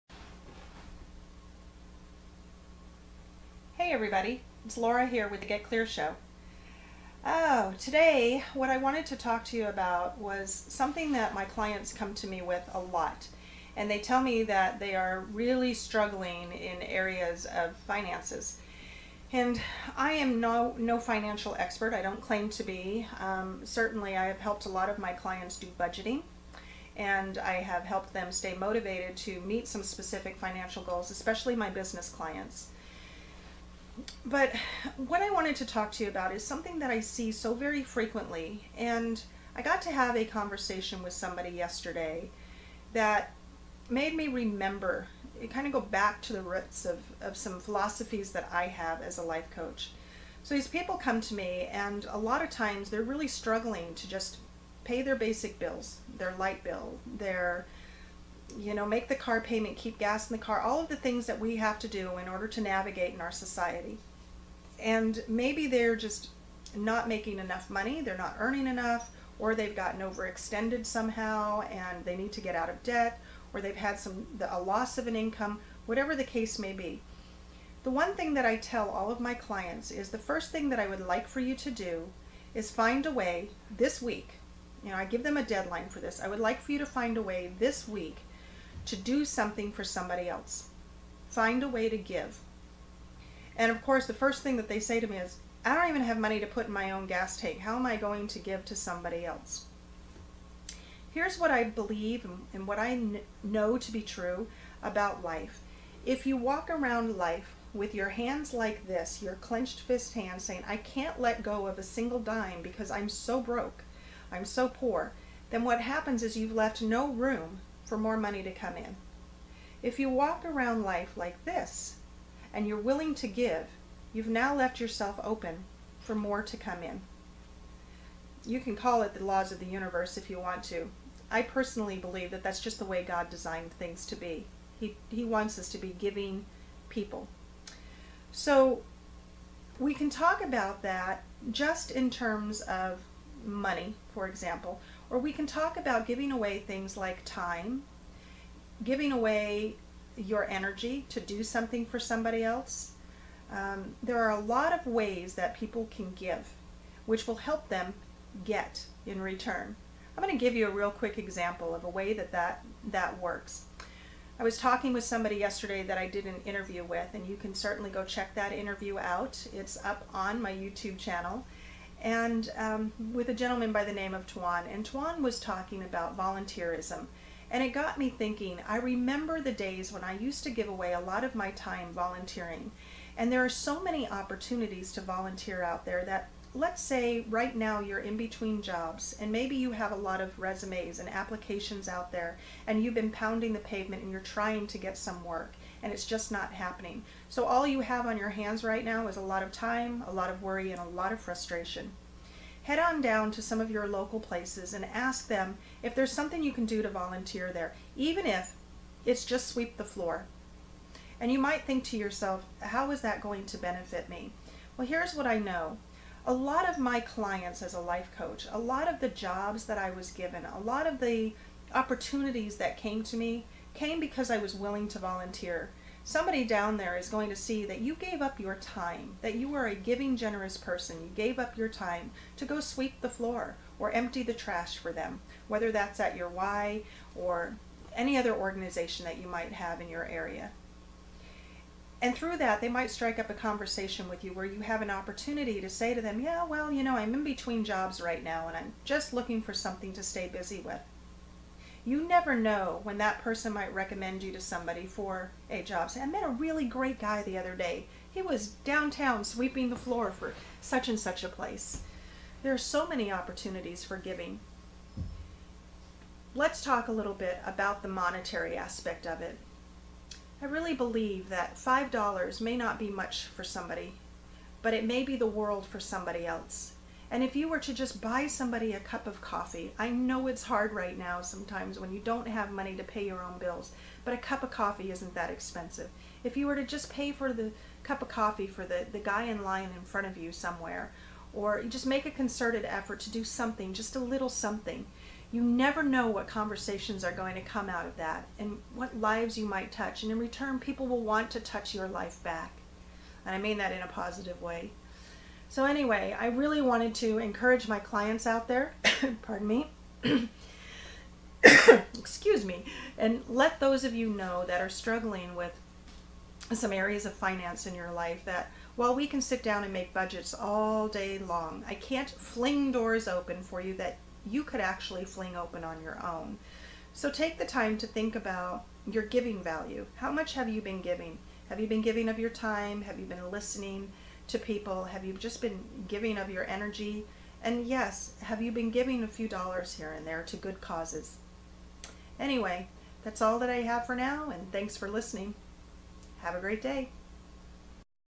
TIRED OF BEING BROKE? Give to Get
Tired of being BROKE? Life Coach, Laura Wing, explains the secret to getting a job or more money, by giving.